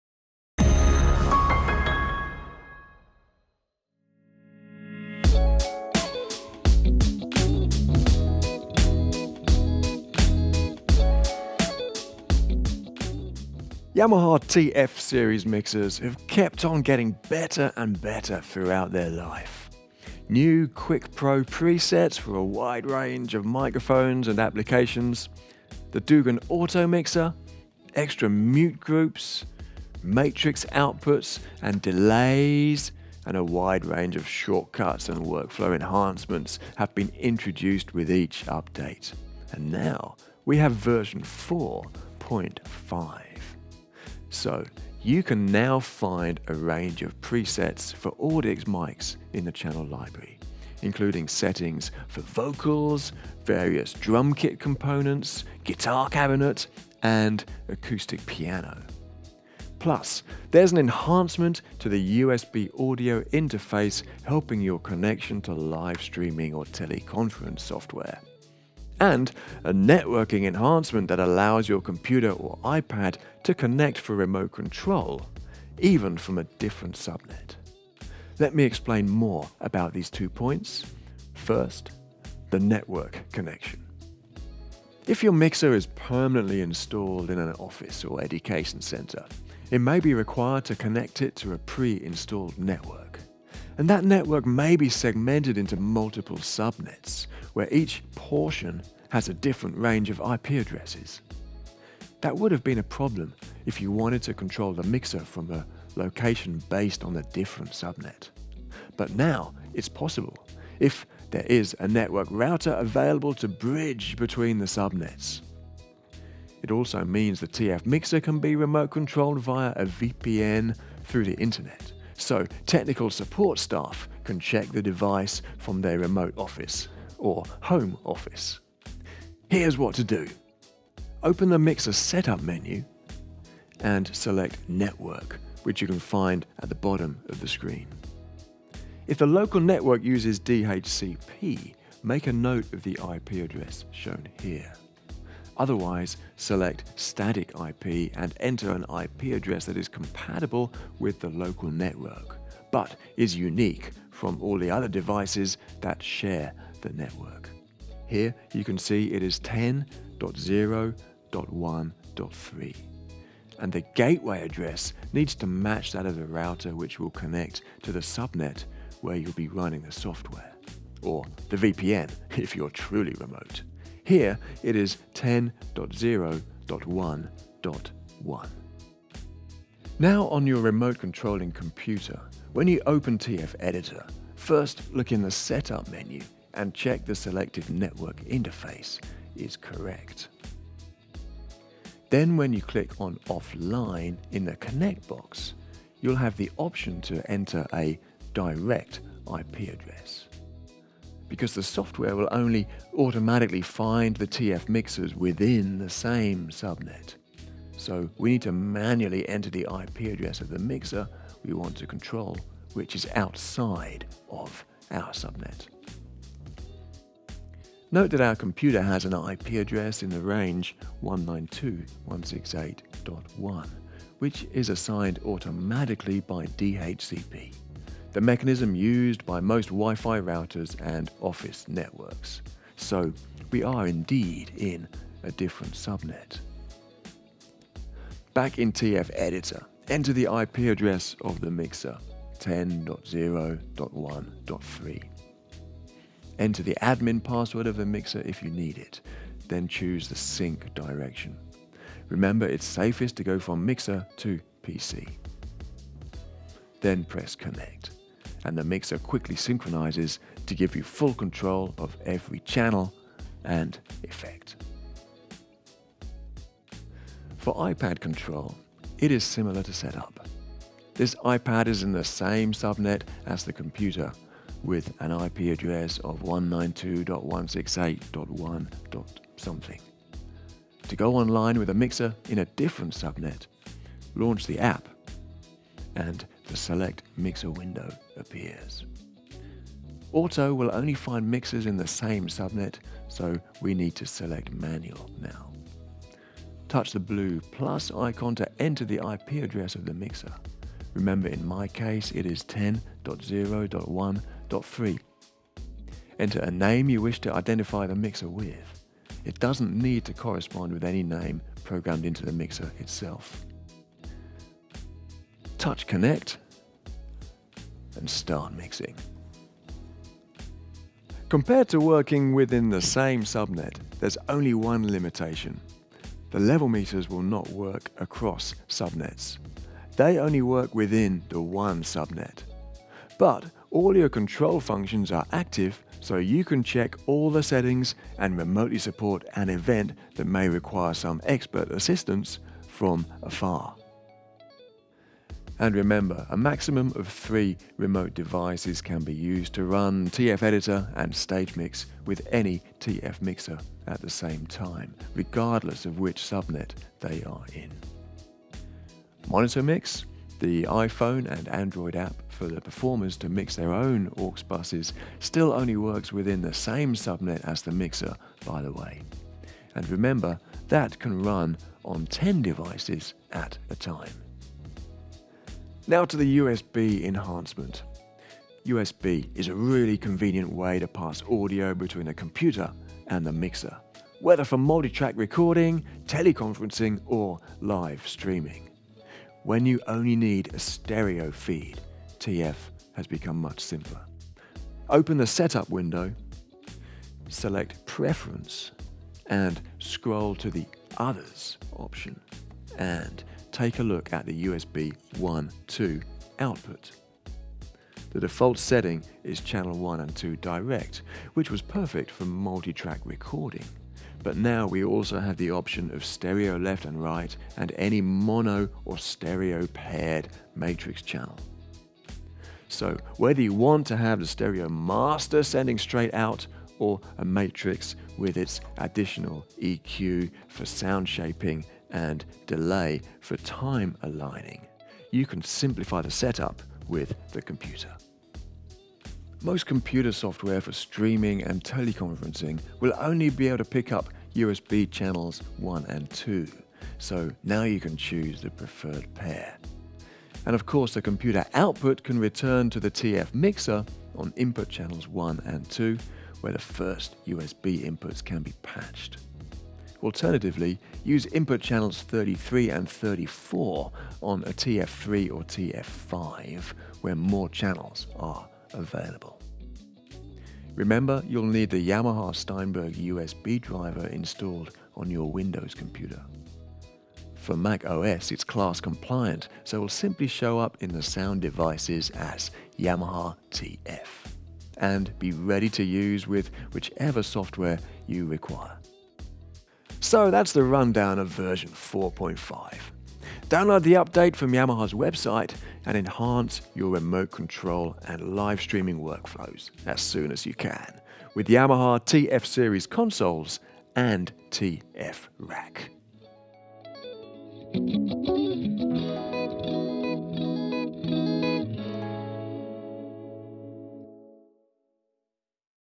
Yamaha TF Series: Version 4.5 Update
Yamaha TF series mixers have kept on getting better and better throughout their life. And now, the TF series has been updated to V4.5. This update brought us Audix mic presets as well as USB and networking enhancement. In this video, we will focus on the USB and network functions.

The subtitles are available in the following languages:
English, 中文, Español, 日本語, Deutsch, Français

You can find out more about TF series.